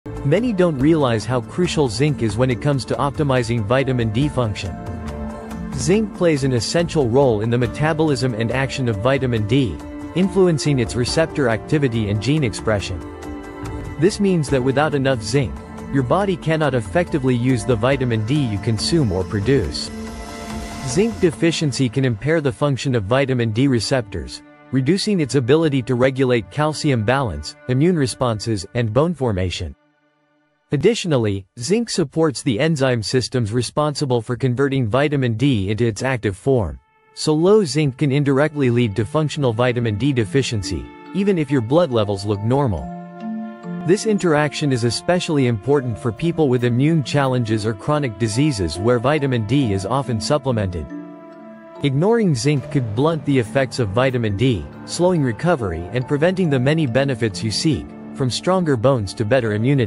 Why mix vitamin D with zinc!
The sunshine vitamin, or vitamin D, is considered a hormone with many functions all over the body, from the bones to the brain. But for vitamin D to work efficiently you need adequate levels of zinc. Zinc is involved in the activity of vitamin D receptors, it’s also related to the transformation to the active for of vitamin D. These are some reasons why vitamin D should always be mixed with adequate zinc levels. Welcome to Levitas! 💯